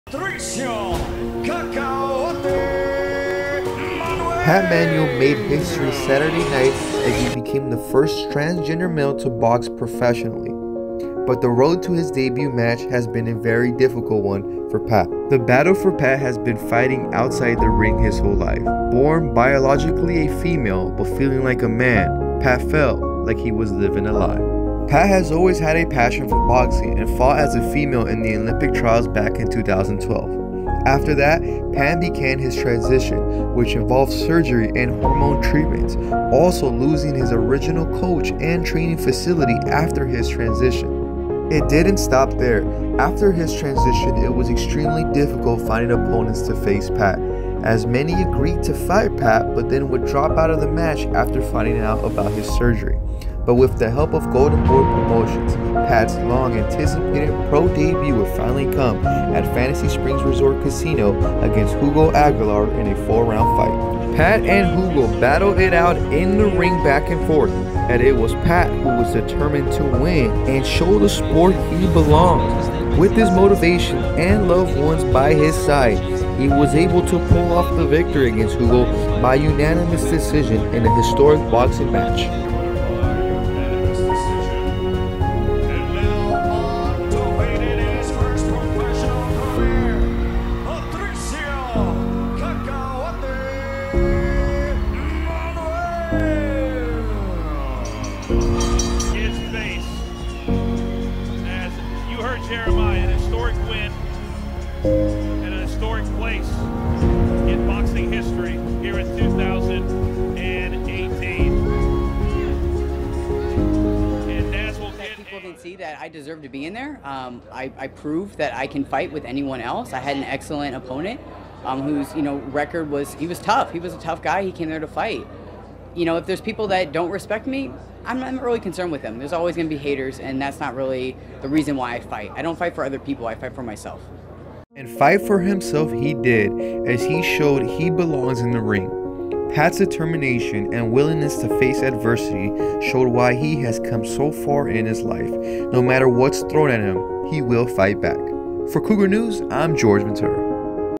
First Transgender Male To Box Professional In Boxing - Pat Manuel
Pat Manuel became the first transgender male to box professionally in the United States. The journey to his debut has not been easy as he lost his original trainer and training facility after his surgery.
Pat box as a woman back in 2012 in the Olympic trials, check out the full story to see Pat's journey from transitioning to the boxing ring for his pro debut.